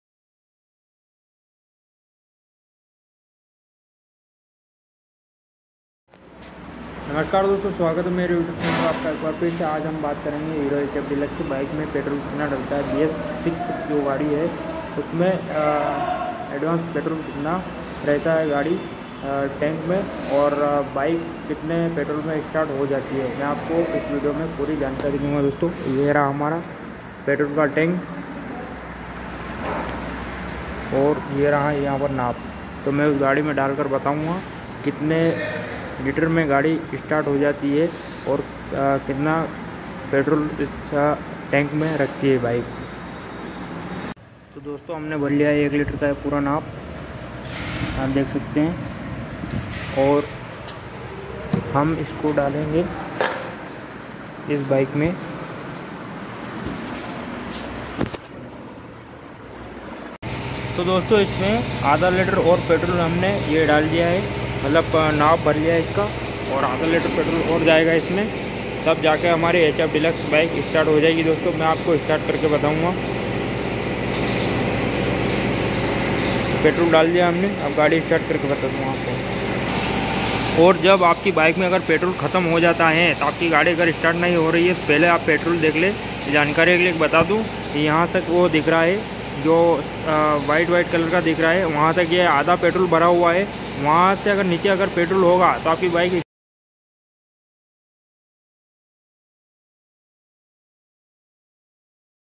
Hero Hf Deluxe Bs6 मे कितना पेट्रोल रखना पड़ता हे puri jankari is video me
Hero hf deluxe bs6 full details
hero hf bs6 me kitna petrol rakhana padta he
Hero HF deluxe bike Mein Kitna petrol rakhna padta hai